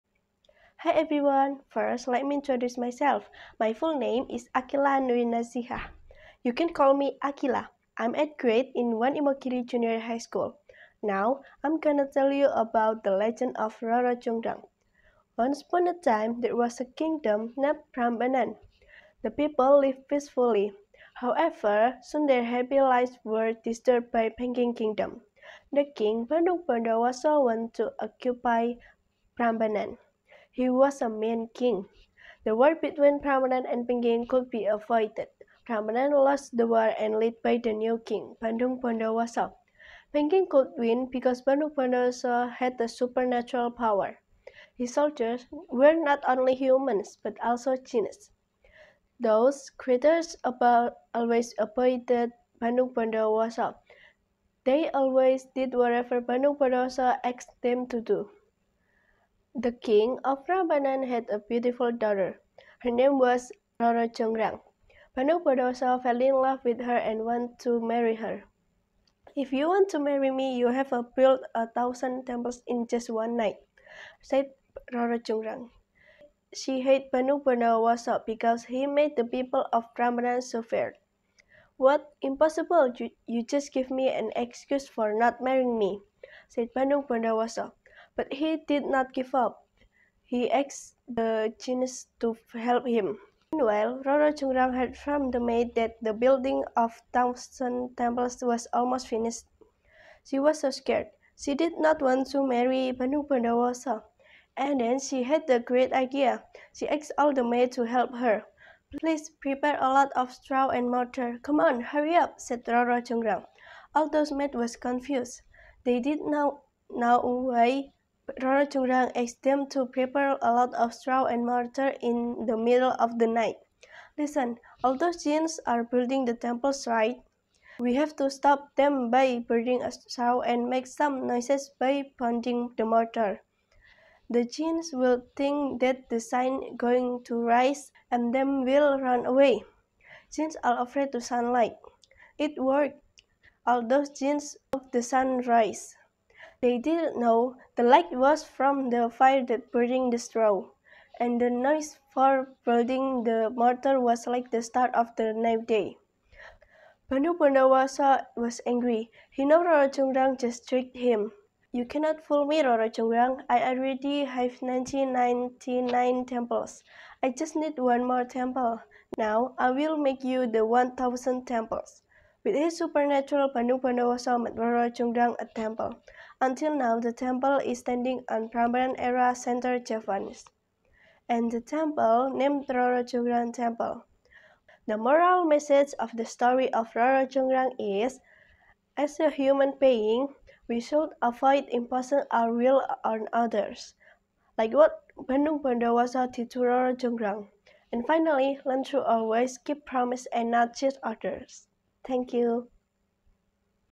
STORY TELLING #62: SMP N 1 IMOGIRI_AQILA NURIY N
This video tells a story entitled "The Legend of Roro Jongrang"